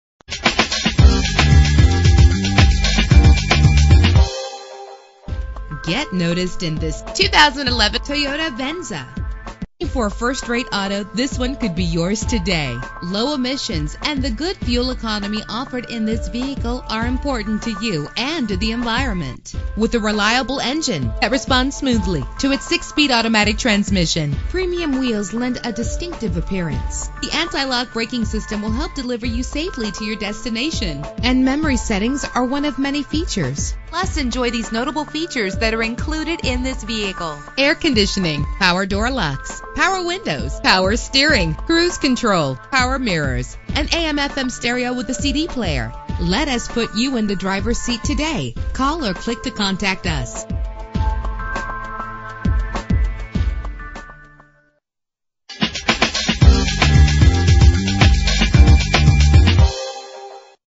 2011 Toyota Venza - MESA AZ
Year: 2011
Make: Toyota
Model: Venza
Trim: V6 4D Sport Utility
Engine: 3.5 liter V-6
Transmission: 6-Speed Automatic
Color: Tropical
Mileage: 15
Address:
6136 E AUTO LOOP AVE